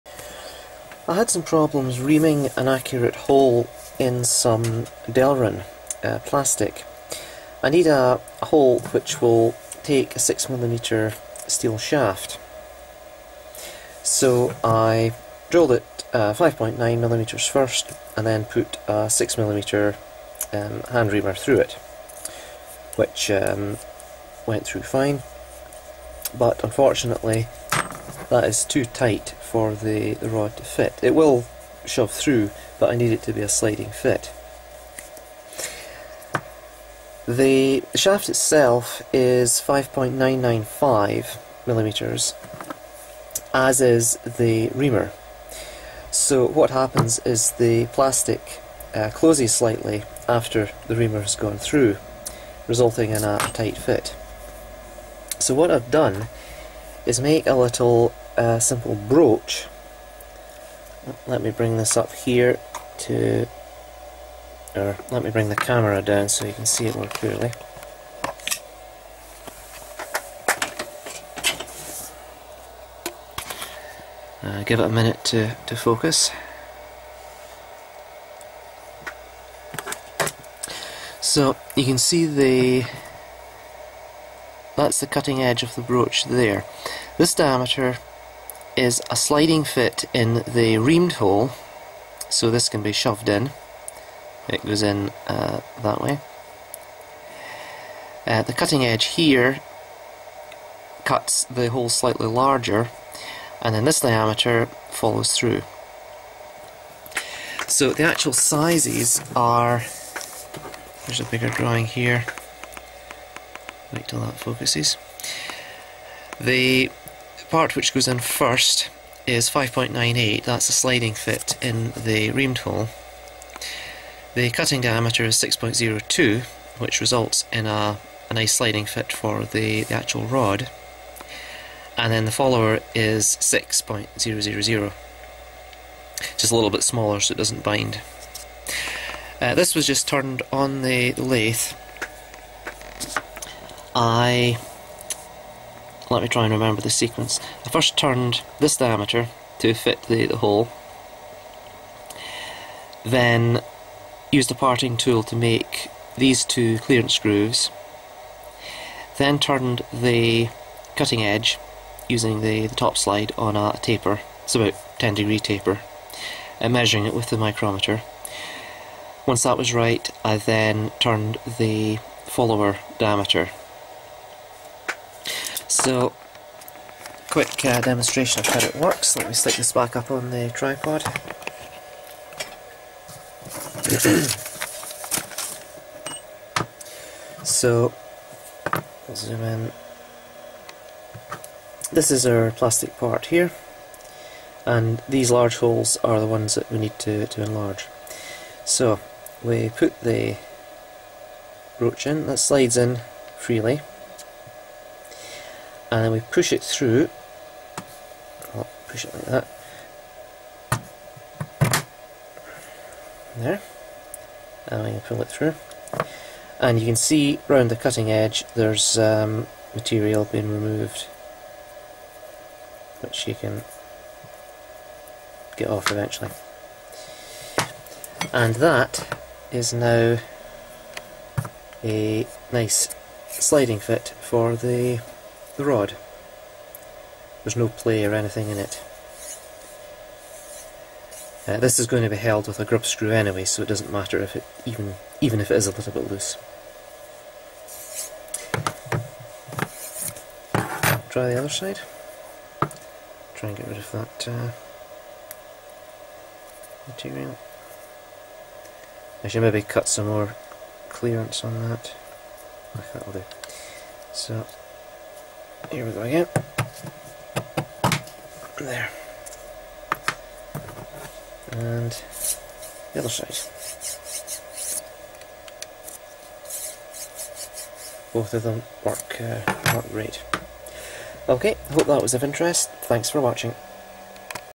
Single-tooth 6mm broach cutter
This is a simple tool for producing an accurately-sized hole in some delrin/acetal parts I'm making. A hand reamer results in a slightly undersize hole, so this is used to enlarge it slightly and provide a sliding fit for the linear rails.

The AxiDraw® computer-controlled drawing machine. A full kit of parts is available to build the machine - please check out for full details and demonstration videos!